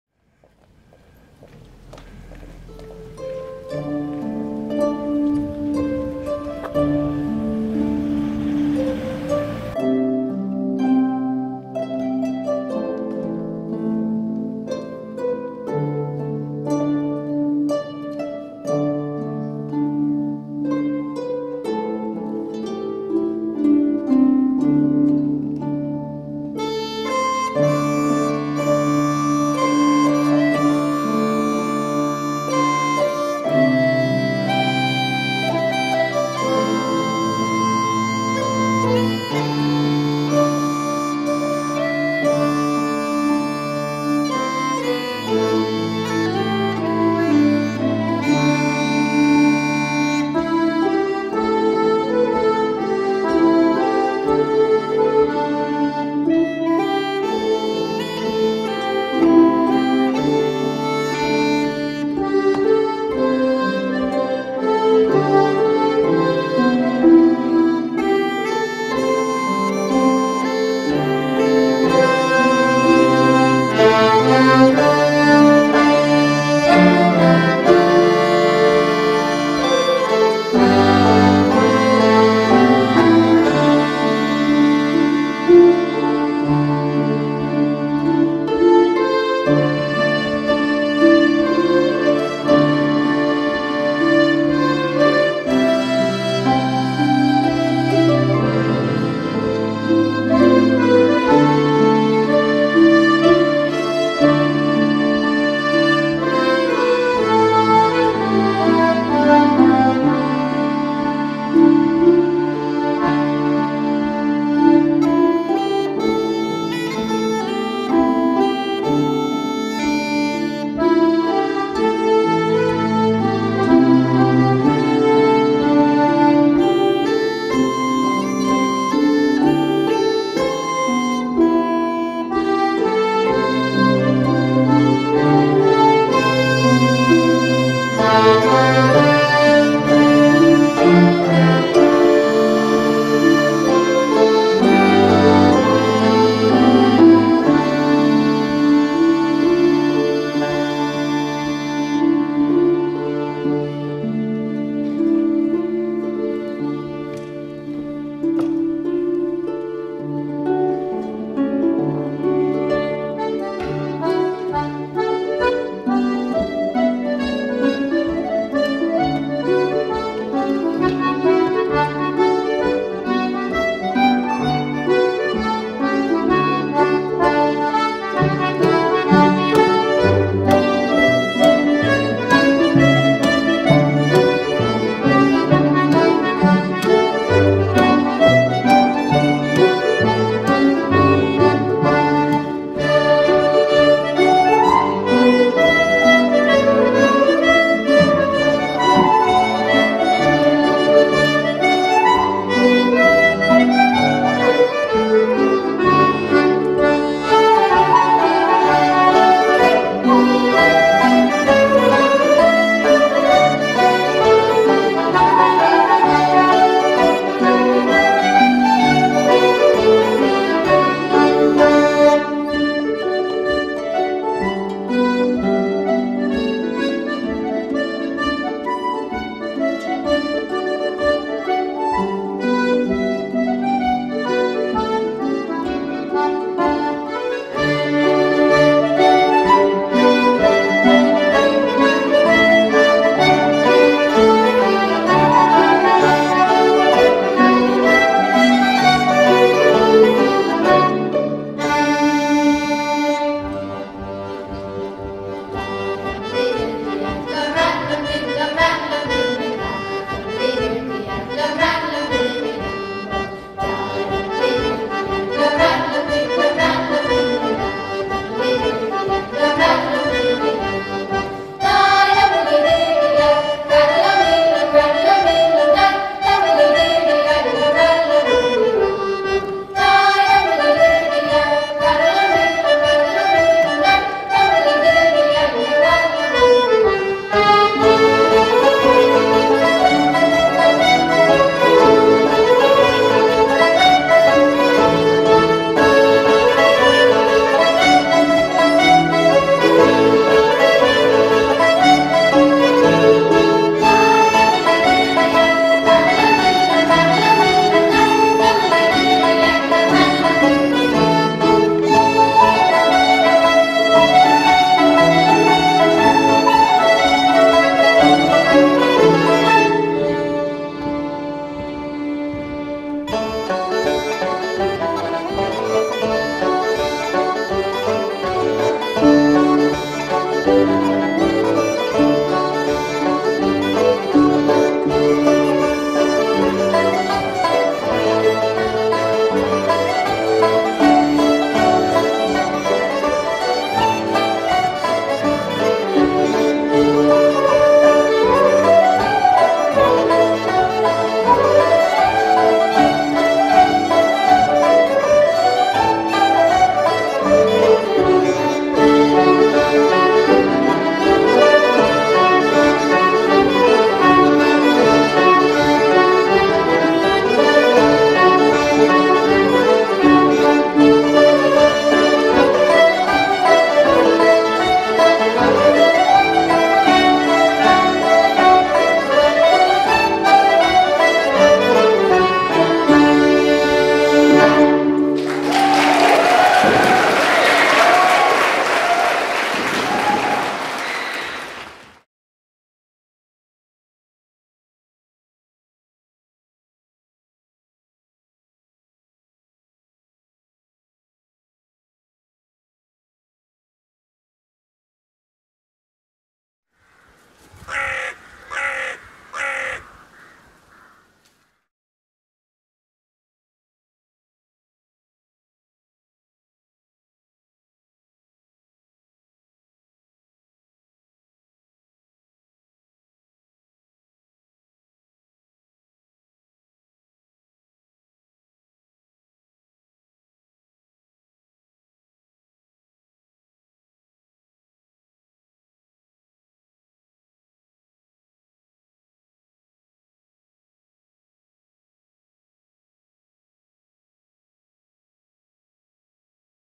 COMHALTAS BALLINSPITTLE U12s - Kinsale
Comhaltas Ballinspittle under 12s perform a medley of traditional Irish tunes at St Multose Church Kinsale, County Cork, on August 29th 2019 as support act for Zetadam Trio from Brittany on their Wild Atlantic Way tour of West Cork on bicycles.
50% of the proceeds from this concert was donated to St Vincent de Paul organisation